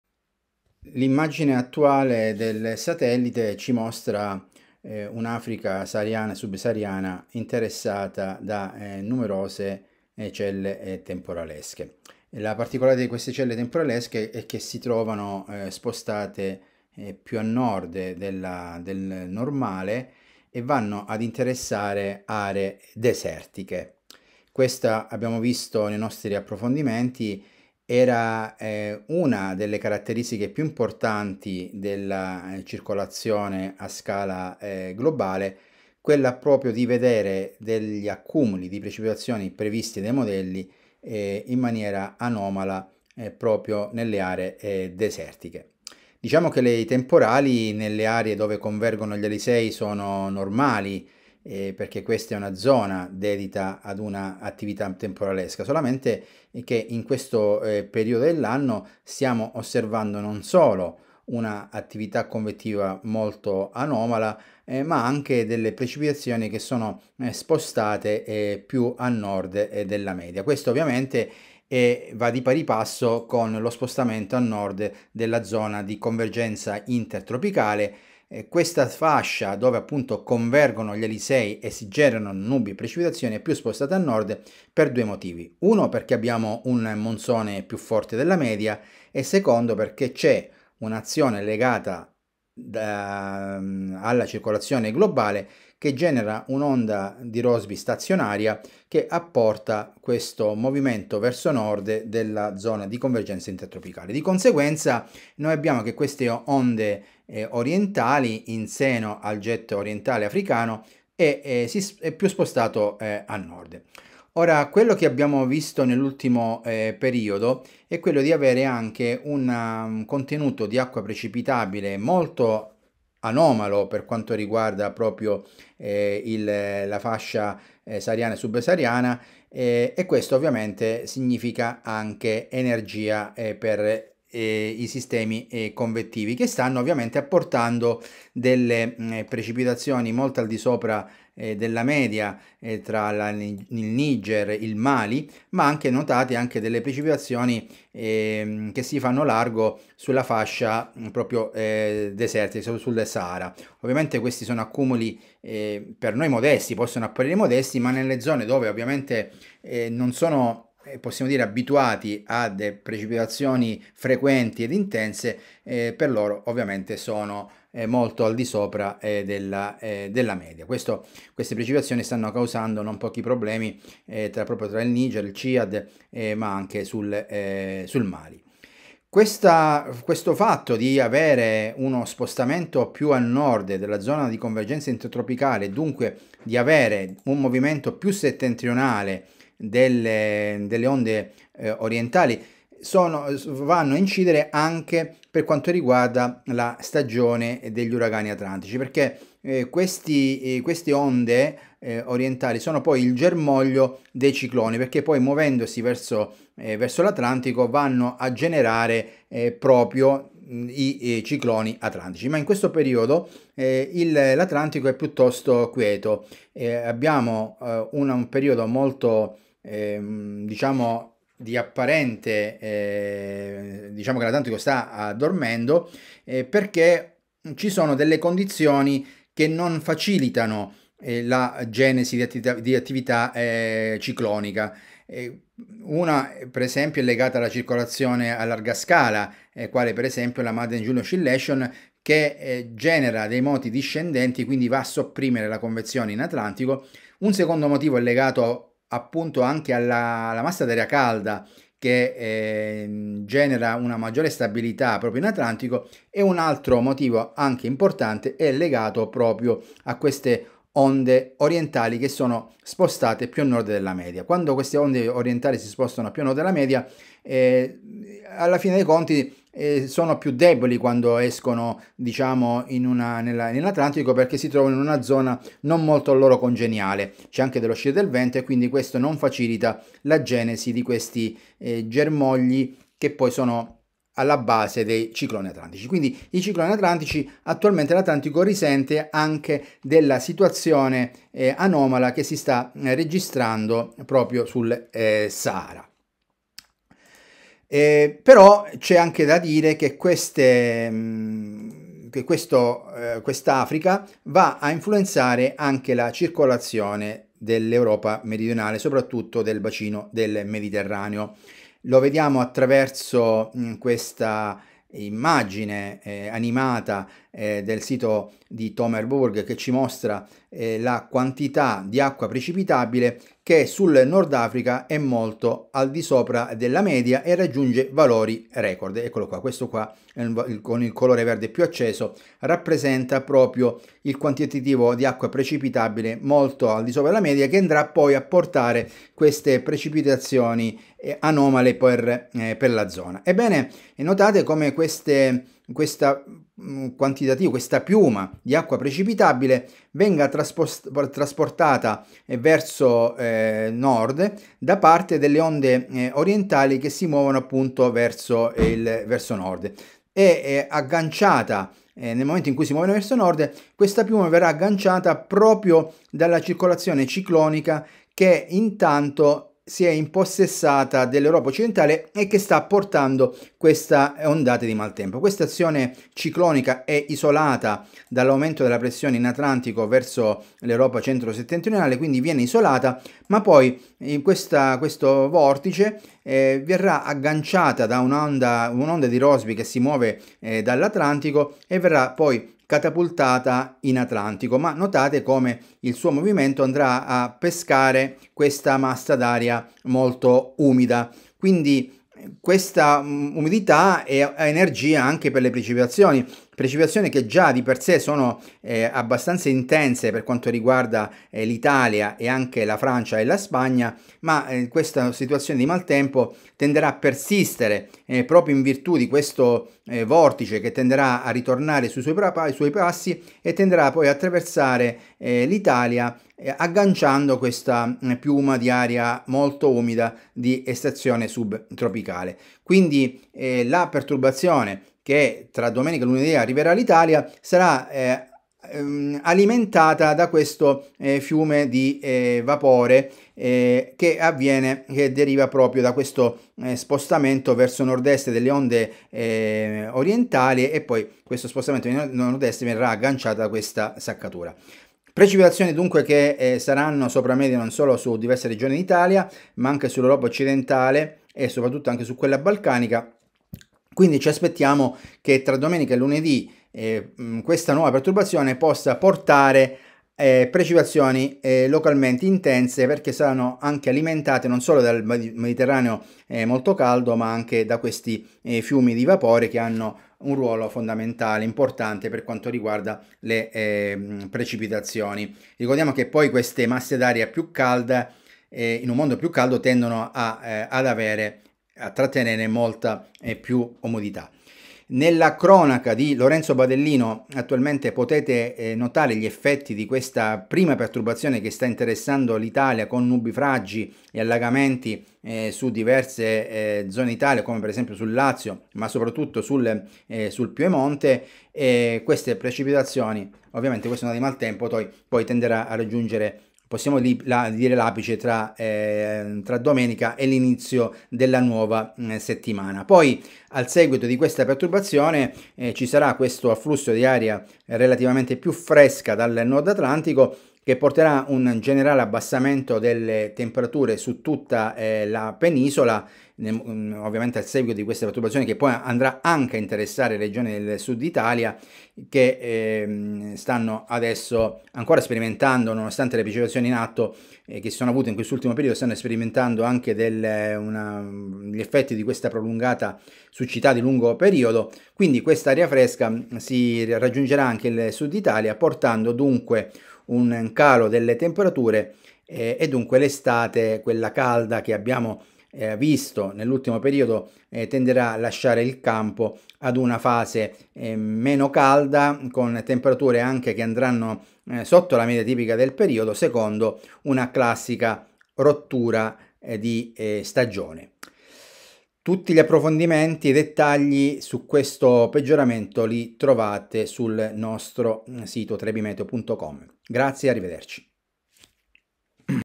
METEO.BURRASCA DI FINE ESTATE, PIOGGE ANOMALE SUL SAHARA E ATLANTICO IN STAND-BY. APPROFONDIAMO
Il cambio di circolazione sull'Europa dà il via ad una seria di ondate di maltempo in questo avvio di settembre. Nel video di approfondimento spieghiamo come le piogge delle nuova settimana siano collegate all'anomala situazione tra Sahel e Sahara.